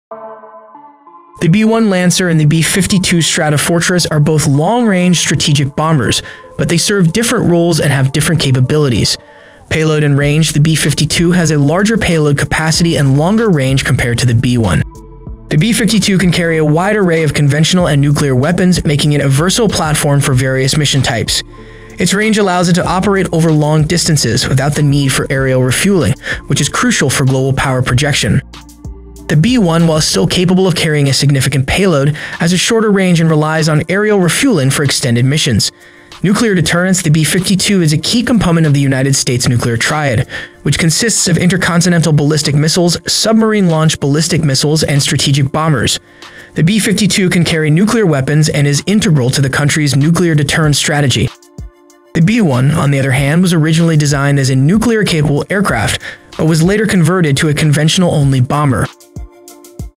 Why Can't the B-1 Lancer Replace the B-52?
The B-1 Lancer and the B-52 Stratofortress are both long-range strategic bombers, but they serve different roles and have different capabilities. Payload and Range, The B-52 has a larger payload capacity and longer range compared to the B-1.